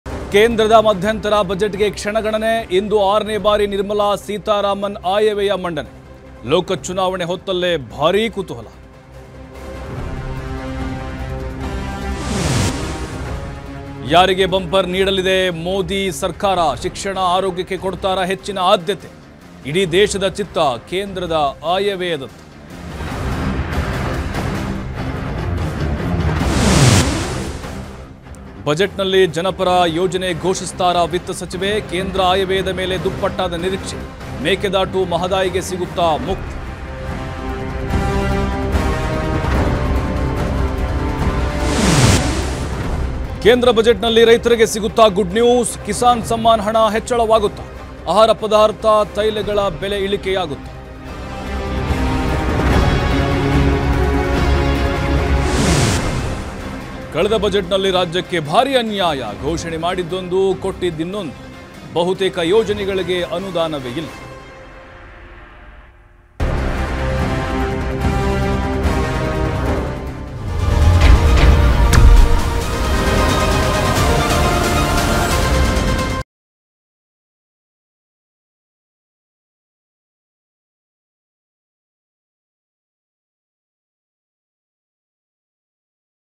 9AM Headlines : Budget 2024 will be presented by Finance Minister Nirmala Sitharaman | Tv5 Kannada
9AM Headlines : Budget 2024 will be presented by Finance Minister Nirmala Sitharaman | Tv5 Kannada